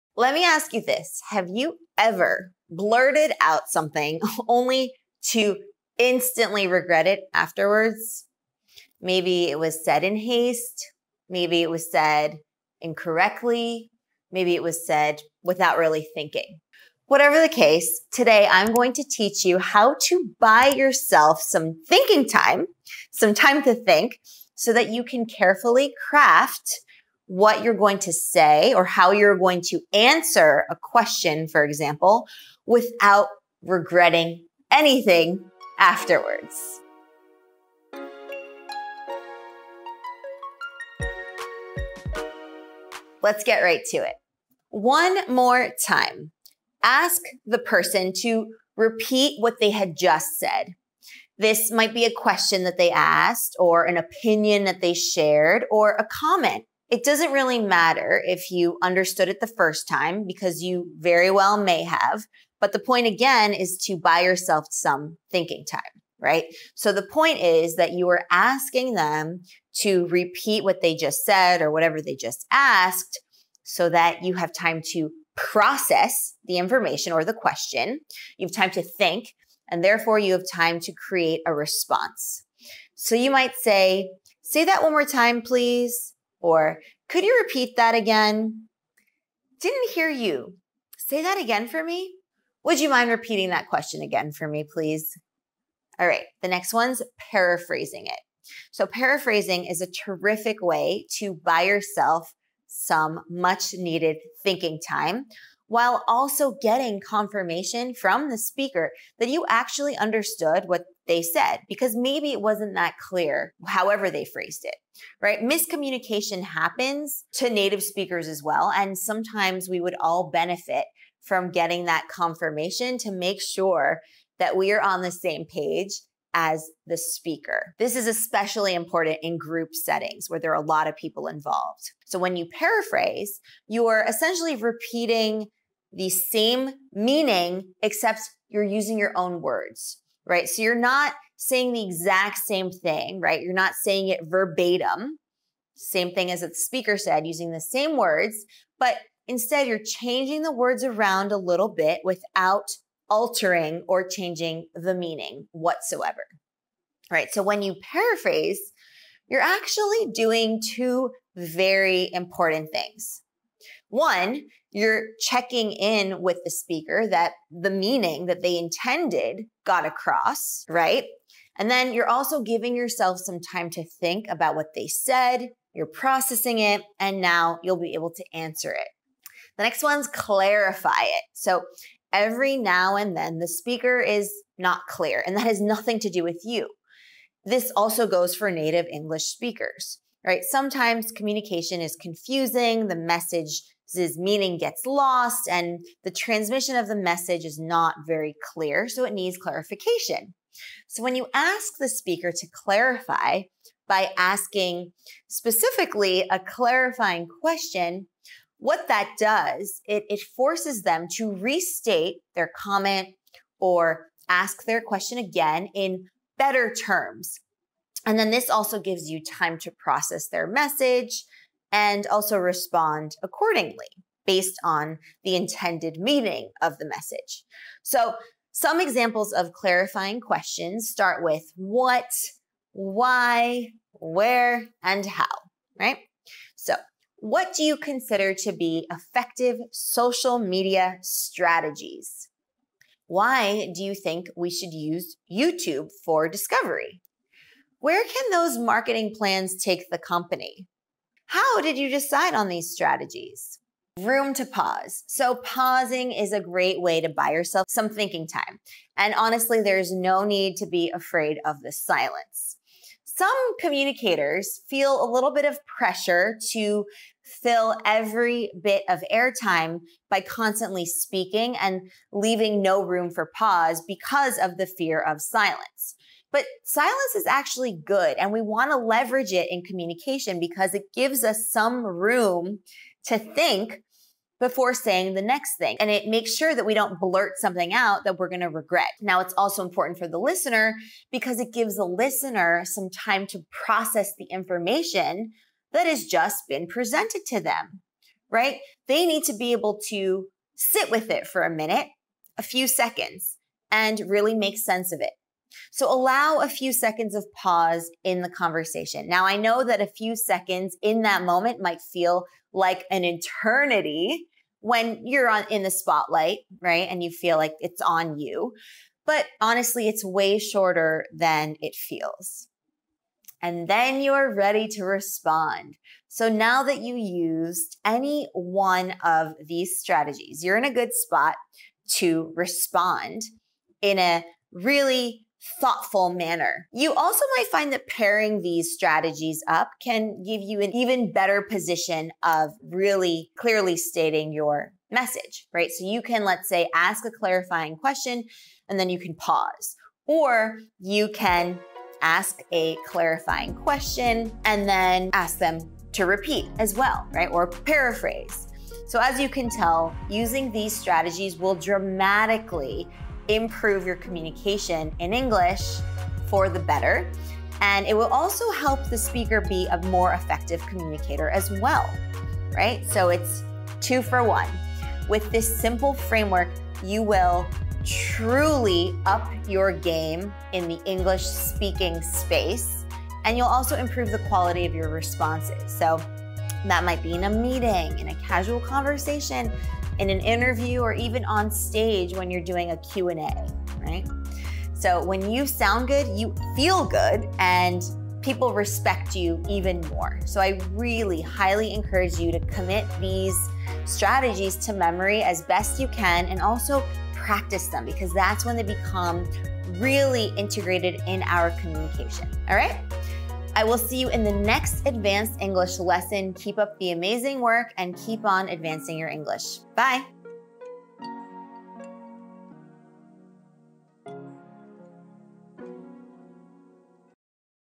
How to Think Before You Speak Out Loud In English
Learn how you can buy yourself some thinking time to process the information and formulate a good response. These are English communication strategies for Advanced English learners. If you want to speak English confidently, you'll want to think before your speak out loud.